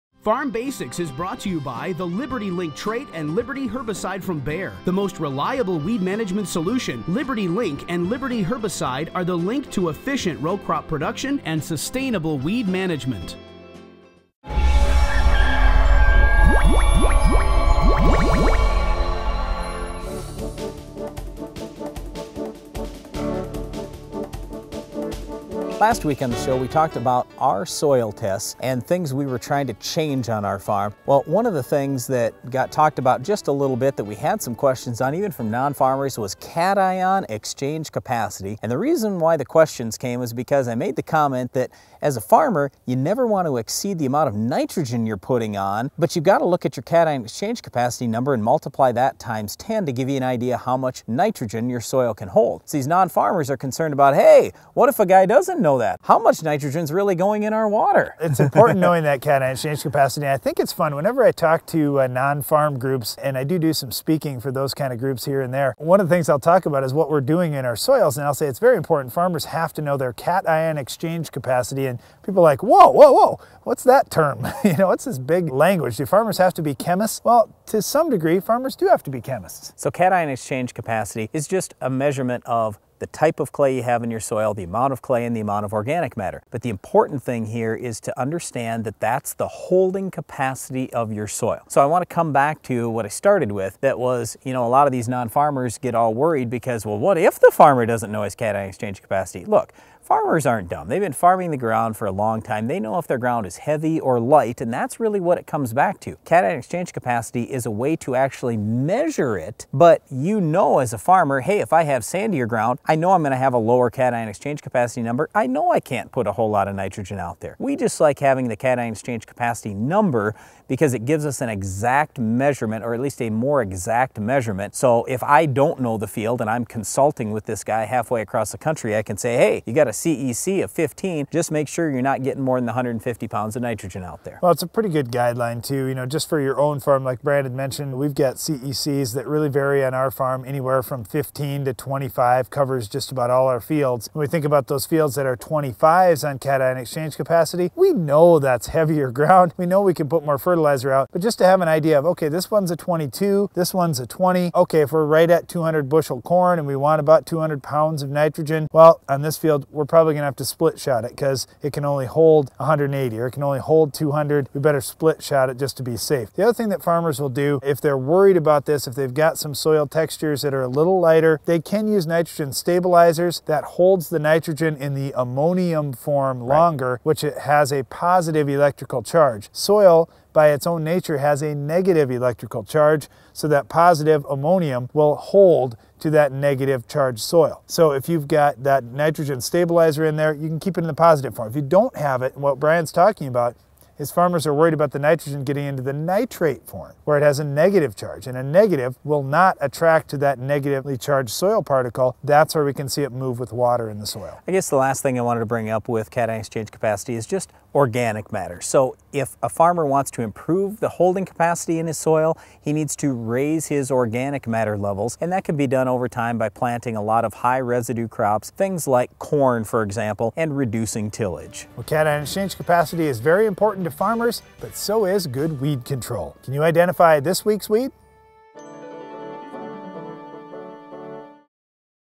Farm Basics # 818 - CEC (Air Date 12/8/13)
Brian & Darren go over CEC (Cation Exchange Capacity).